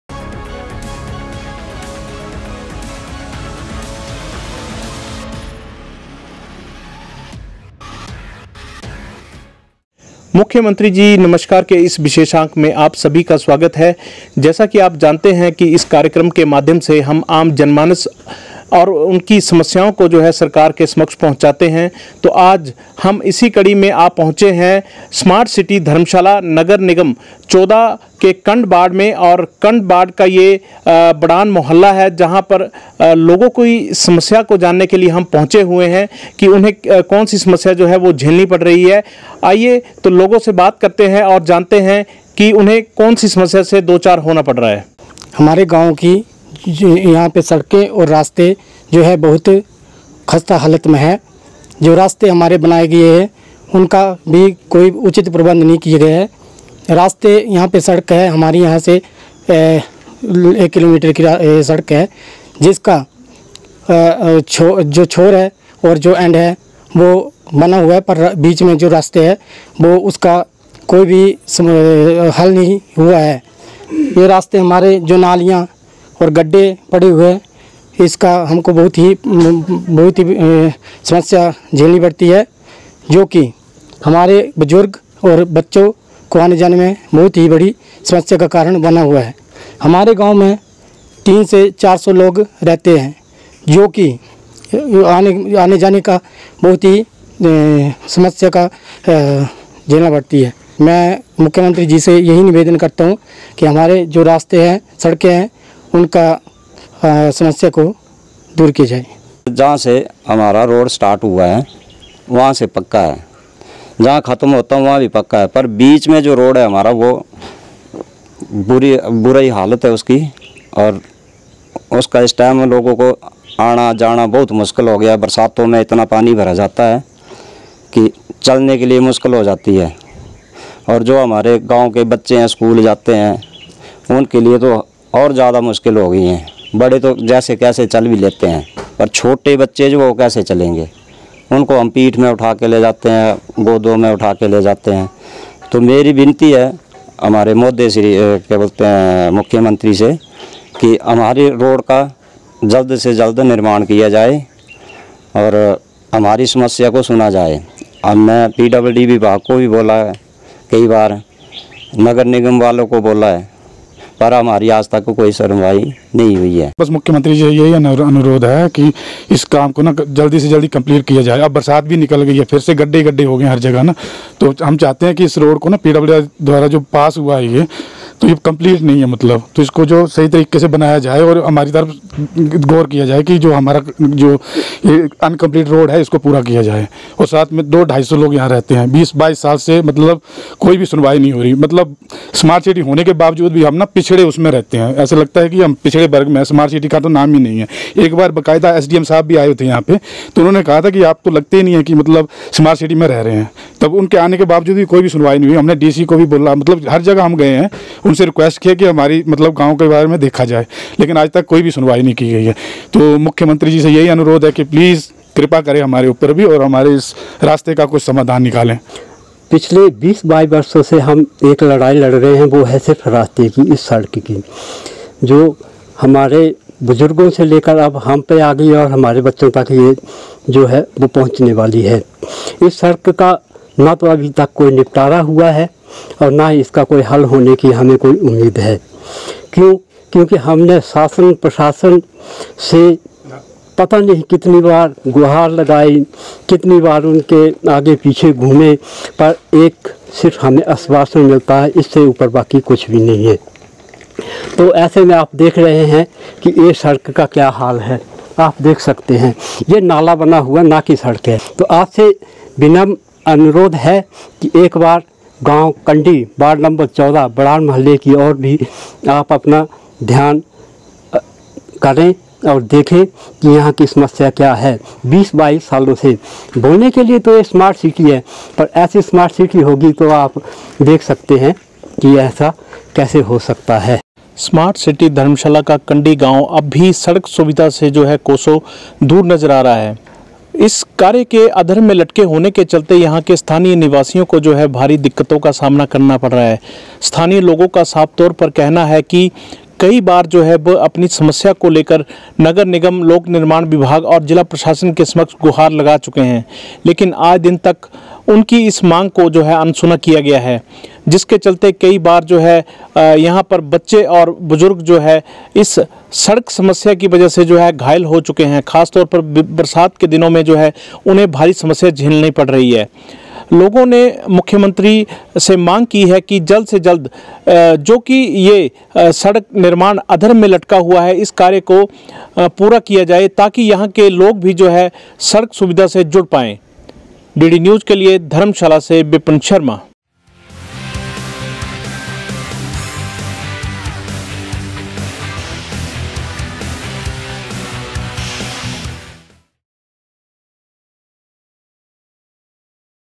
स्लग मुख्यमंत्री जी नमस्कार I
धर्मशाला में अतिक्रमण के चलते लोग बदत्तर सड़क का इस्तेमाल करने को मजबूर I